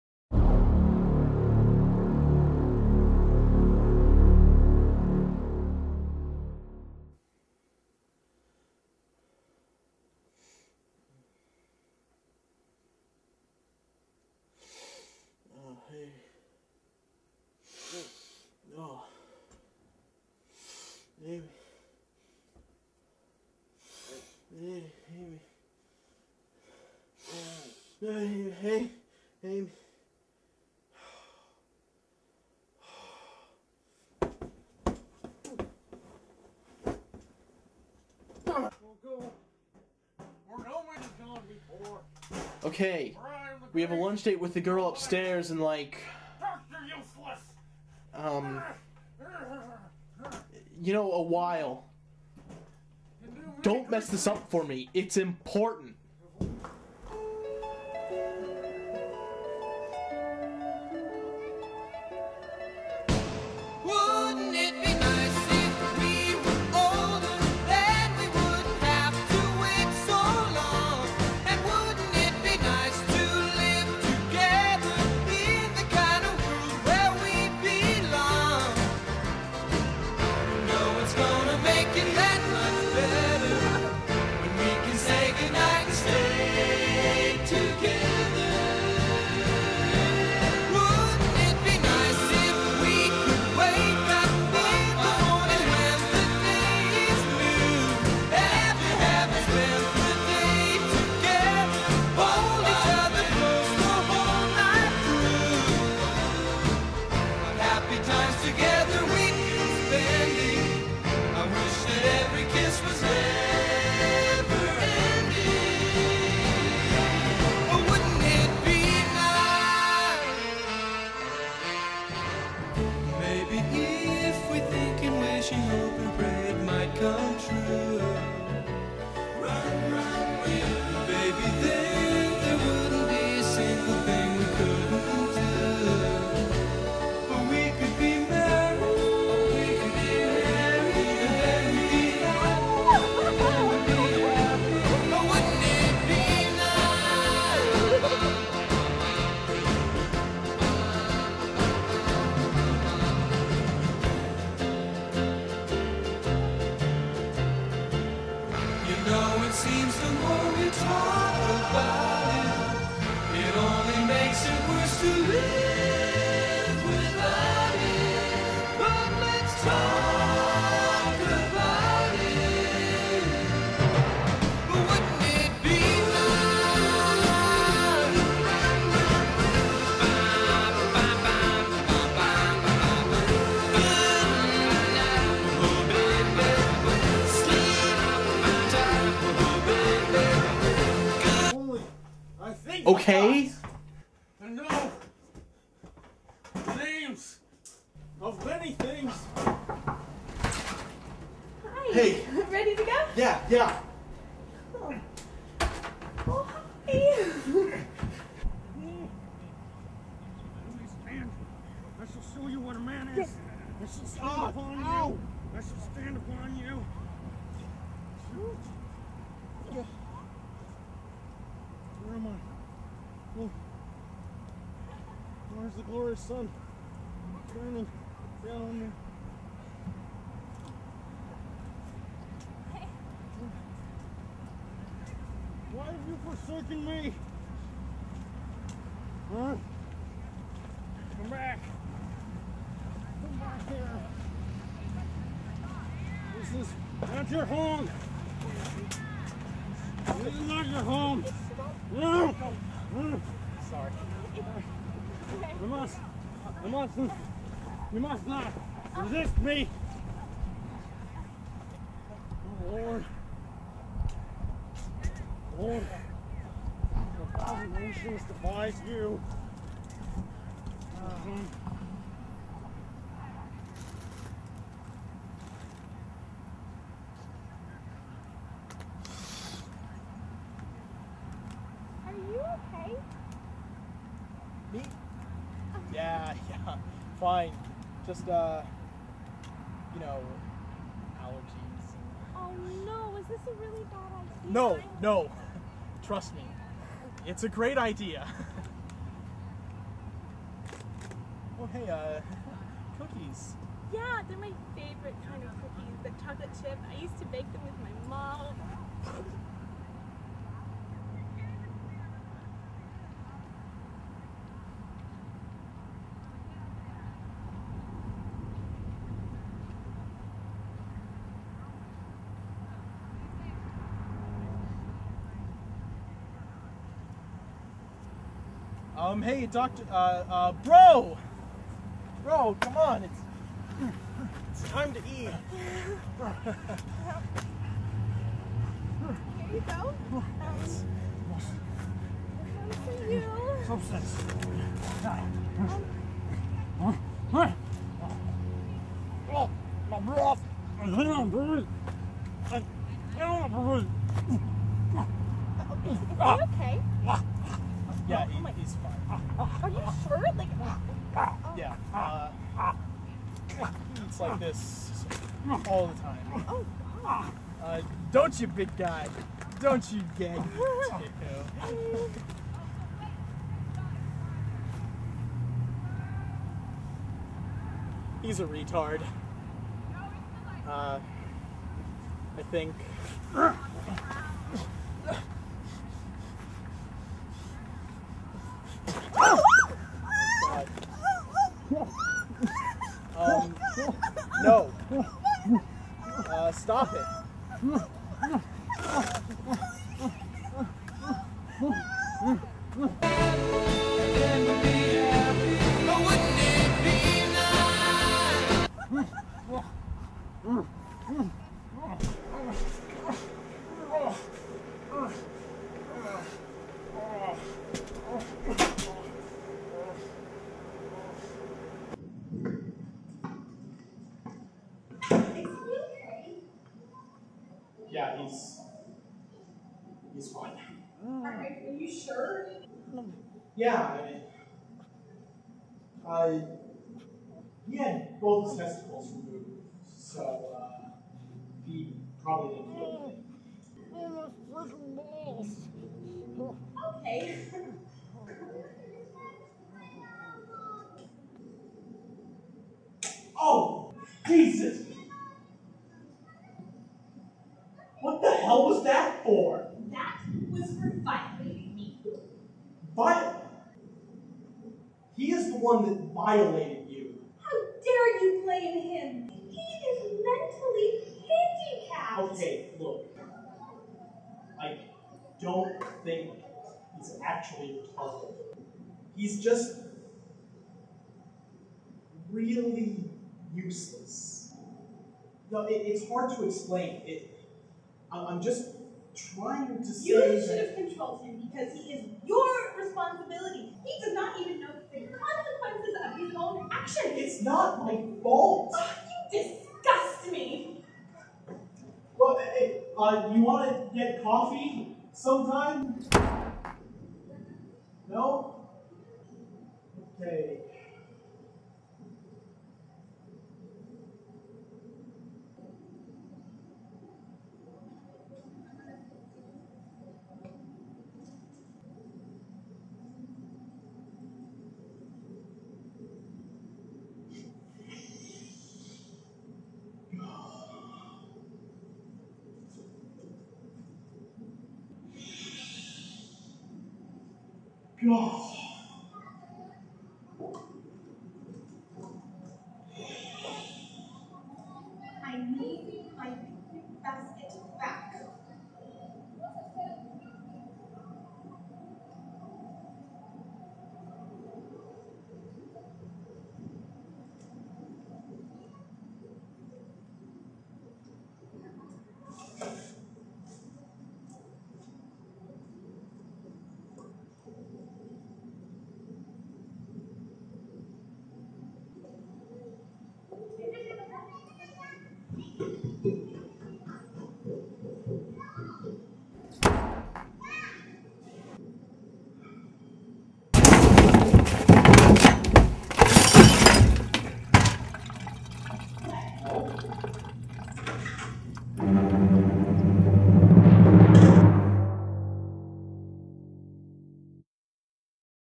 Clockeye and Doctor Useless Episode 7
"Pic-a-nic Basket"

Because it is so nice out, Clockeye and Amy go for a picnic, with romantic results.  Doctor Useless is there, too.